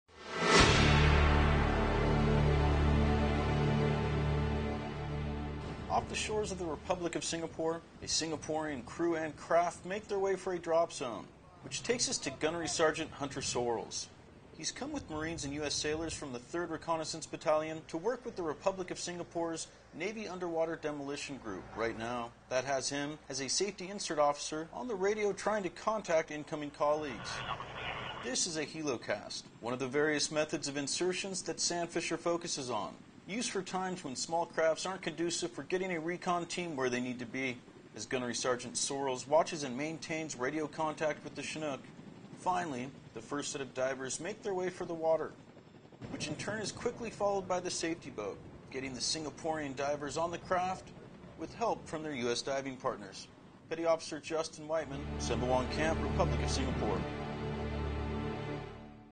Marines conduct helocast with Singaporean Military
Divers from Singapore's Navy Underwater Demolition Group train alongside their U.S. Marine counterparts by participating in helocast training during Exercise Sandfisher 2012. Petty Officer 3rd Class Justin Whiteman has more.